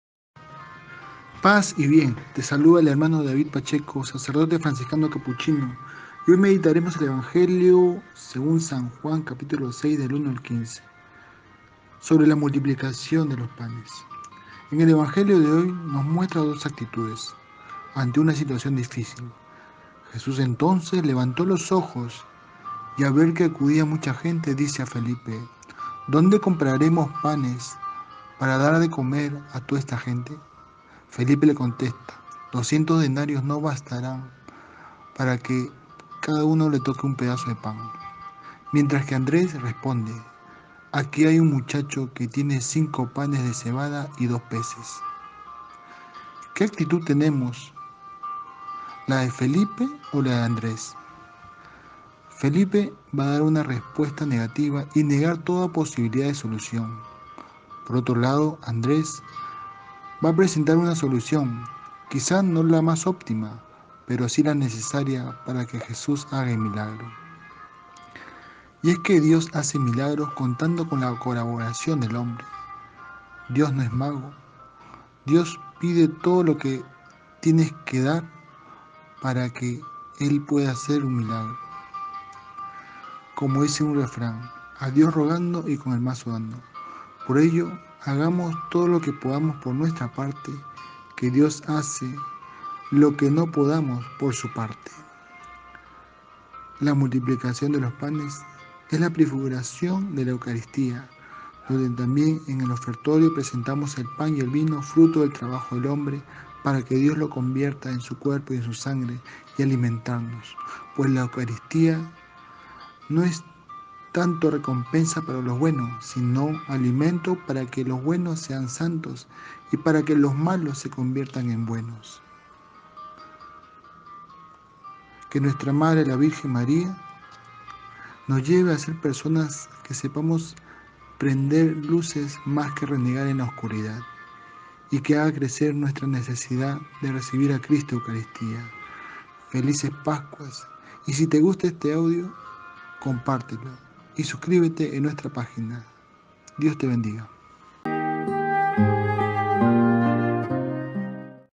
La multiplicación de los panes
Reflexión sobre el evangelio  de hoy viernes 13 de abril, por el Hno. David Pacheco Neyra, OFM cap.
Cita bíblica: Jn 6, 1-15